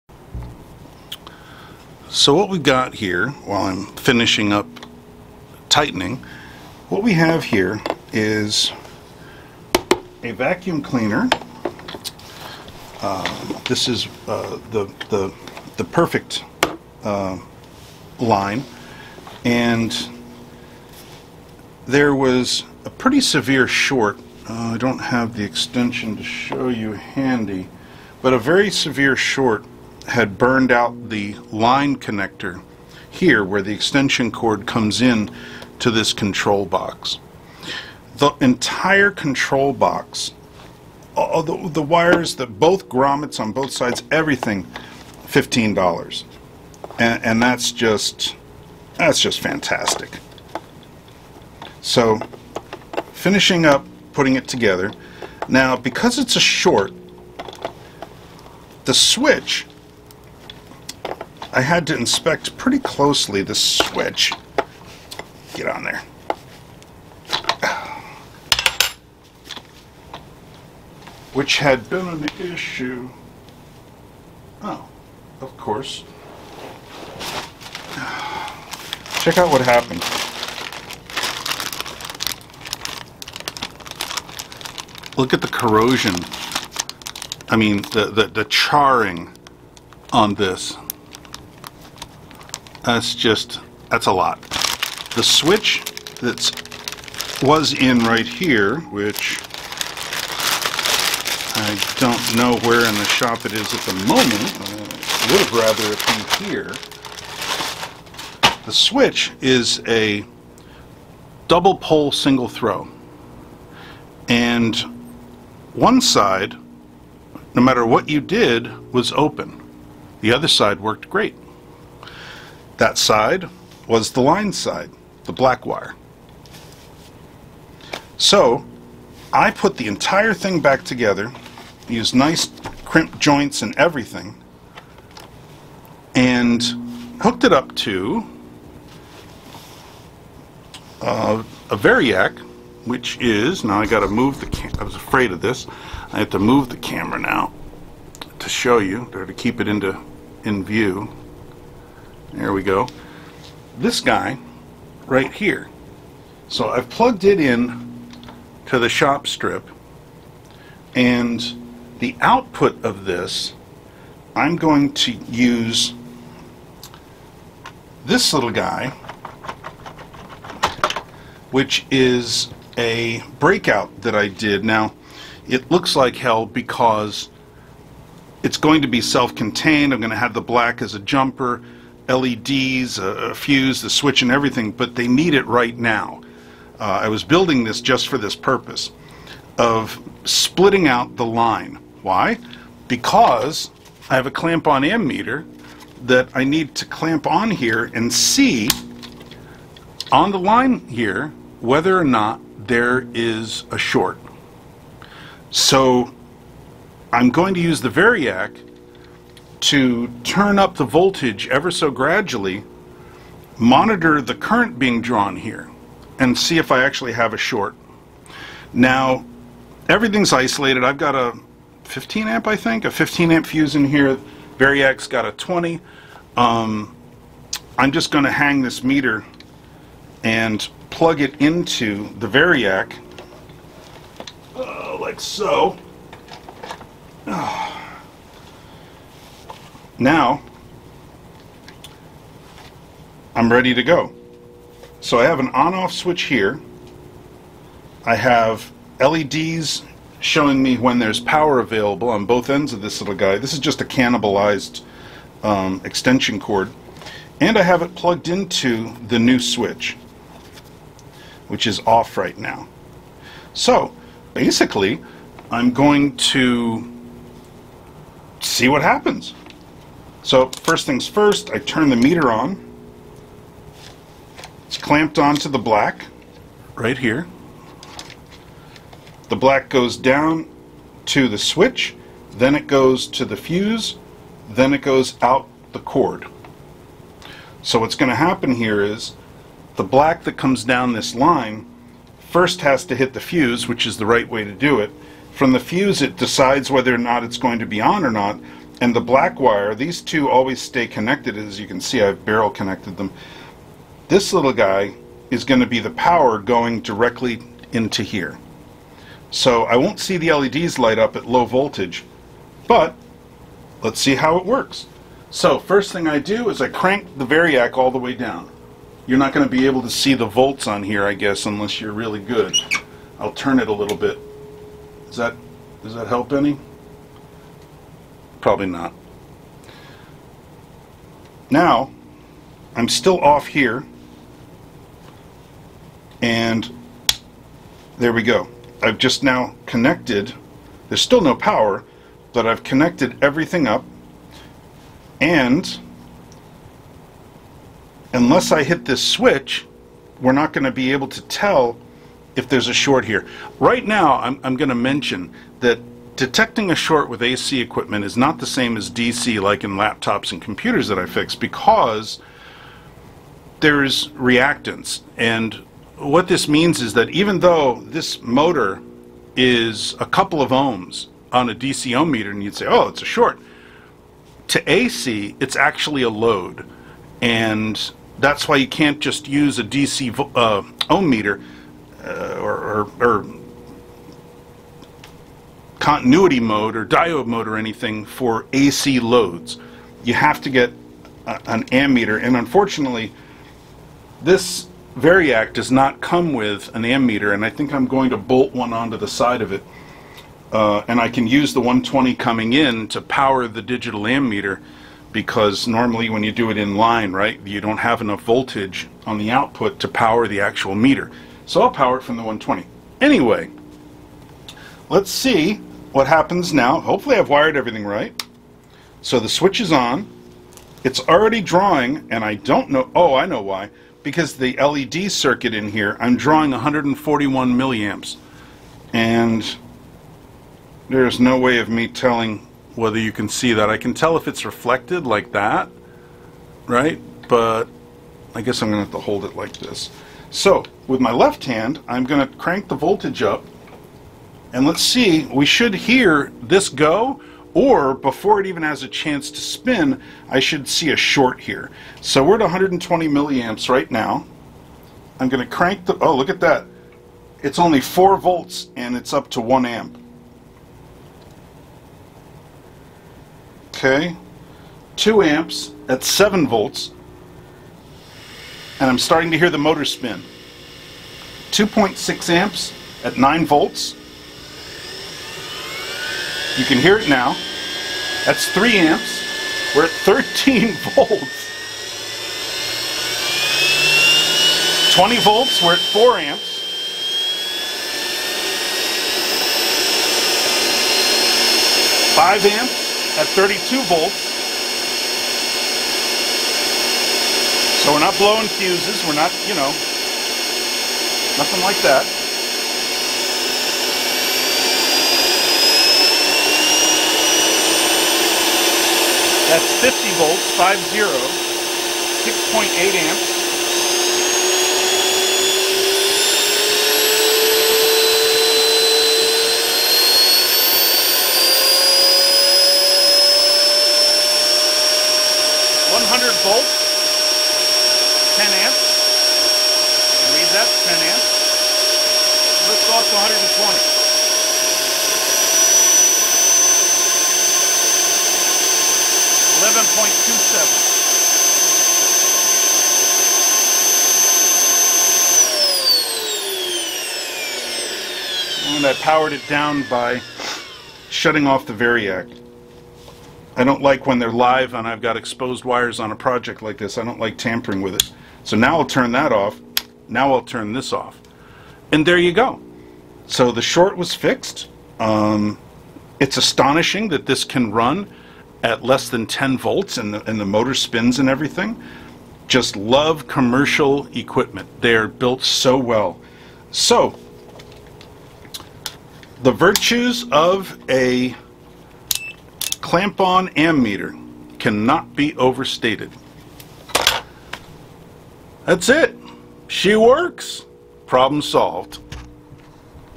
#001 Testing for short on commercial vacuum cleaner
This is a "Perfect" brand 10l model that came to me dead. It had a nasty plug on the control box which showed burns. The DPST switch on the controller was bad (one side open), so I took a project in progress, hacked it to be useful for now, and used a clamp-on ammeter to detect whether the replacement controller board would work.
After all, was the motor or wiring shorted?
If it was, without a variac, turning it on would blow the controller just purchased for $15.
A Variac alone is not enough-you need to measure current to detect shorts in AC, as an ohmmeter will always show a near short.

Hum removed with AudioDirector.